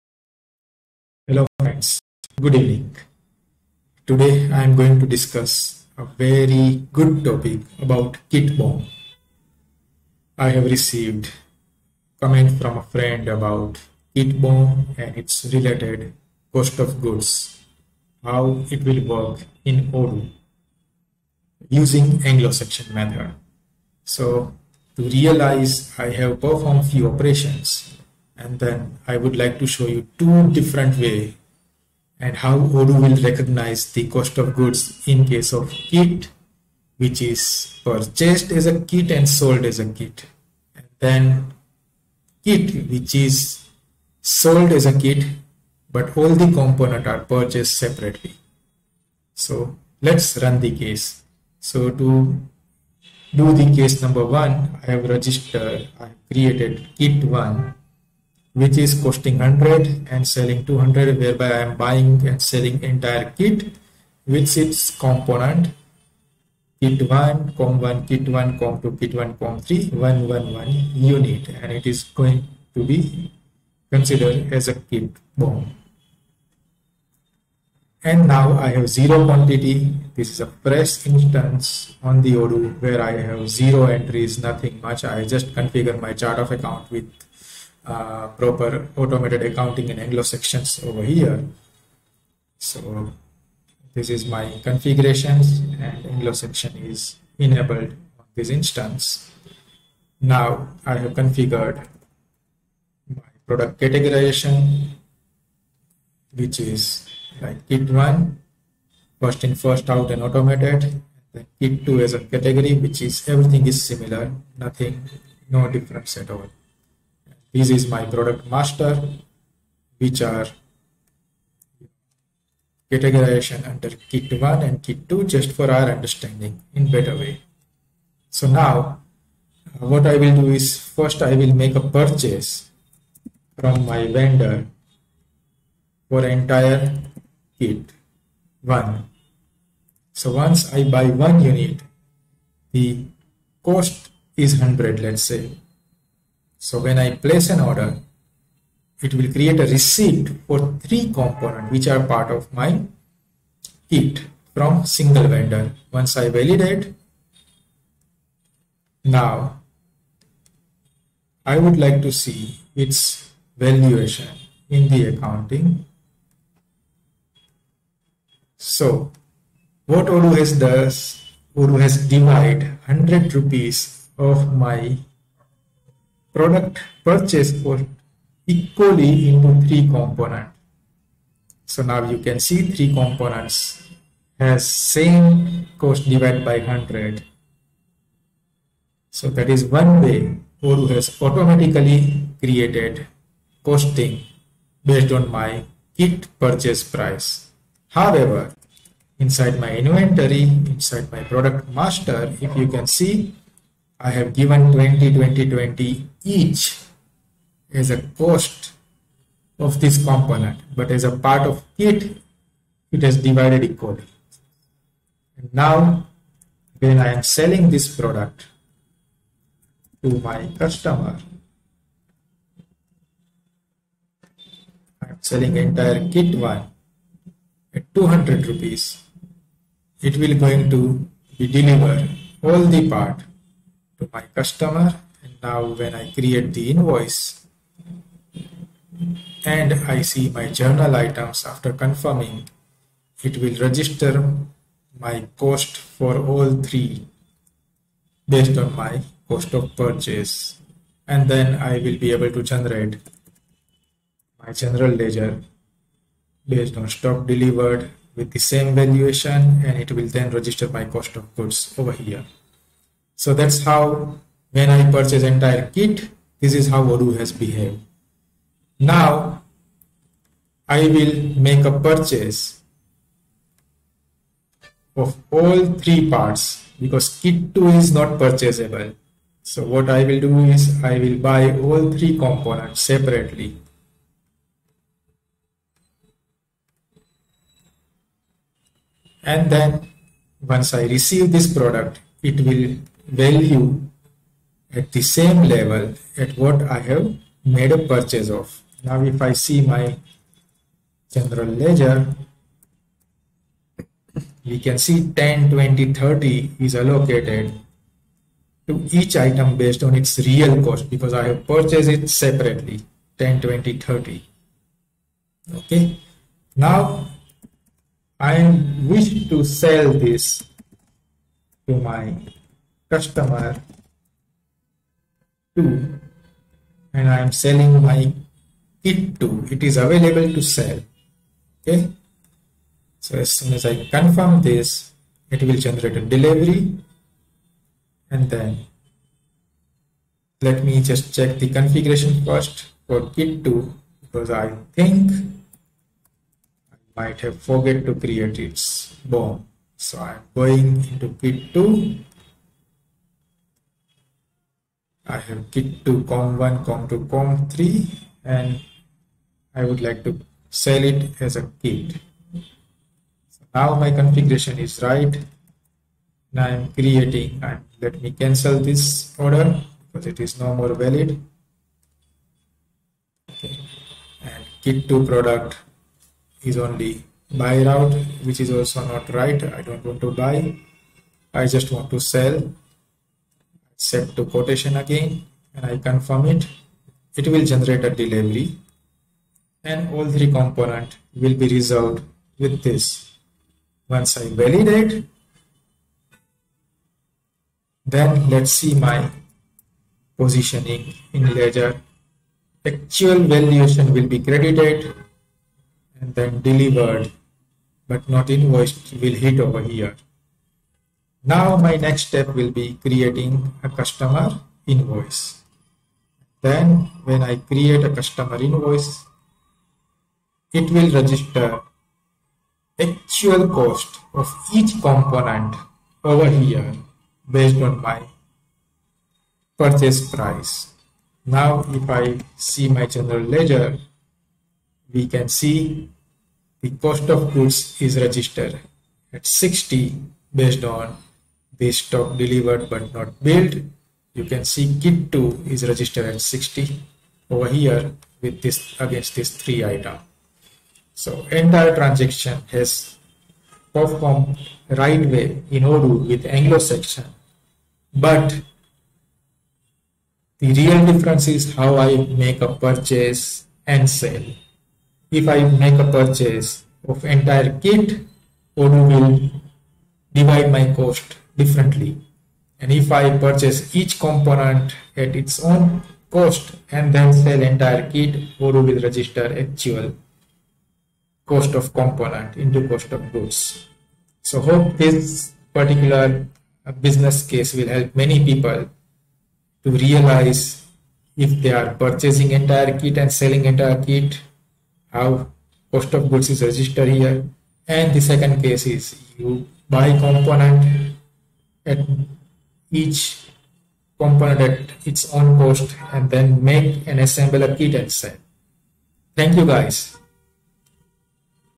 How Kit BOM Purchase and Sales Registers Cost of Goods using Anglo-Saxon Accounting in Odoo
In this video tutorial, you will learn how Odoo recognizes Cost of Goods for Kit BoM while we enabled Anglo-Saxon accounting method. It is very interesting to see two business cases here.

Business Case 1: How Kit Purchase and Sales are registering cost of goods

Business Case 2: How Kit Components are purchased individually and Sold as a Kit to register cost of goods

You will understand Odoo's logic in both the business cases and then you can define your business operations to manage Kit BoM configurations.